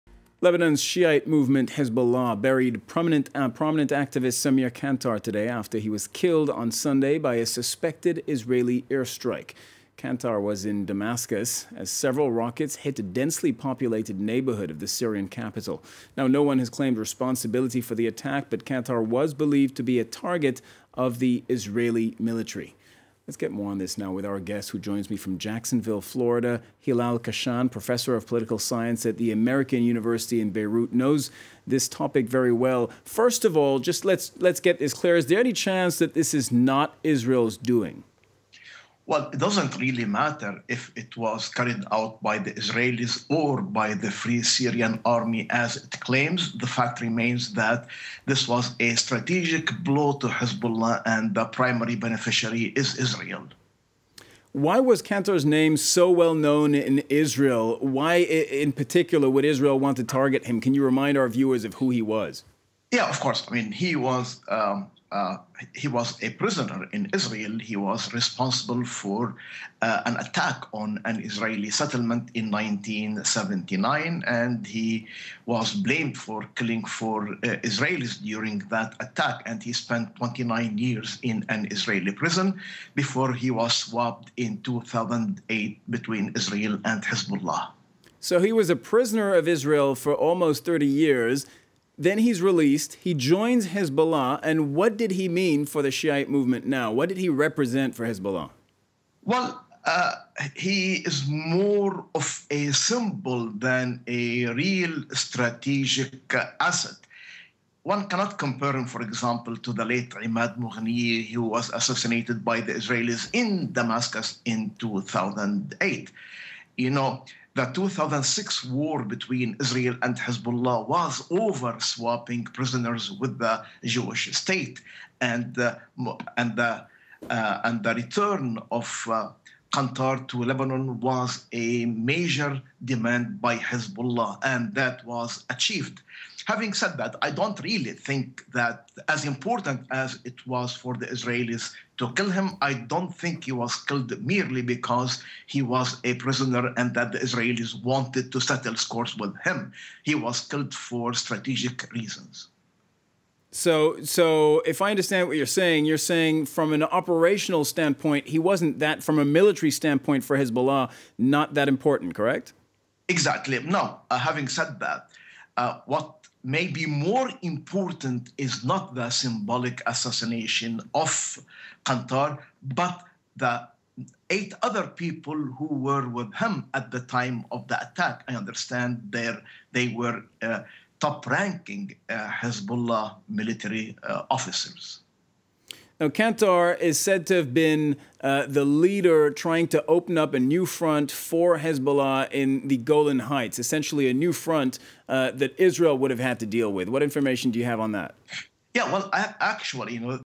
Hezbollah top military leader killed in airstrike: "it doesn't really matter if it was Israel"
Khashan said that the strike in Syria that killed former Lebanese prisoner and Hezbollah militant Samir Al Kantar might have been targeted at other people who were with him in the building. Khashan also said neither Hezbollah and Iran alongside, nor the Syrian regime would retaliate against Israel which is believed to be behind the attack this weekend. Iran wants to keep good terms with the west and the Syrian regime is not really in good shape to respond at the moment.